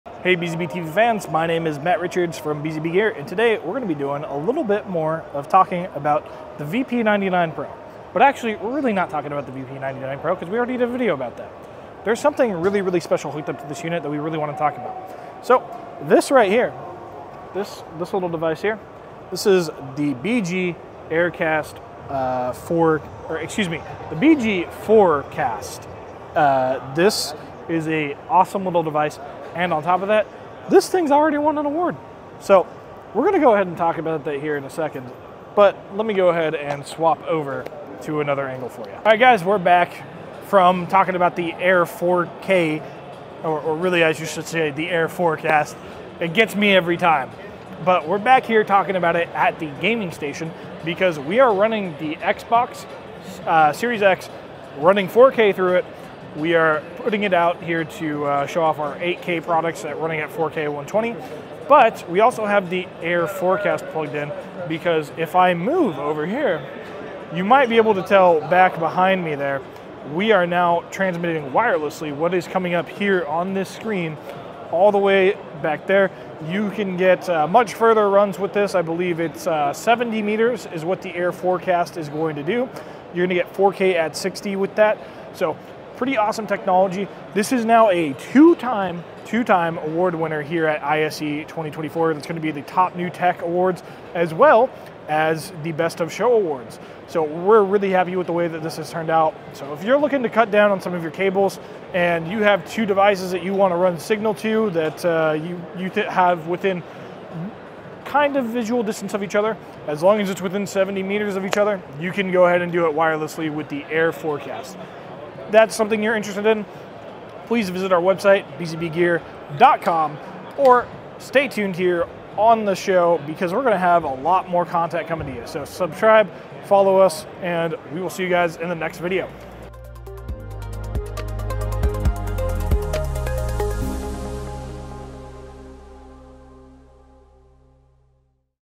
Award-Winning 4K HDMI Wireless Signal Extender from BZBGEAR | ISE 2024
The BG-Air4Kast from BZBGEAR is a brand new award-winning device! It's an HDMI Wireless Extender supporting 4K60 and 5G. We're super happy to announce this device won two awards at ISE 2024: Best of Show by Residential Systems and Top New Technology by Commercial Integrator and CE Pro. Leverage the powerful ipcolor STREAM technology and extend your HDMI signals wirelessly today!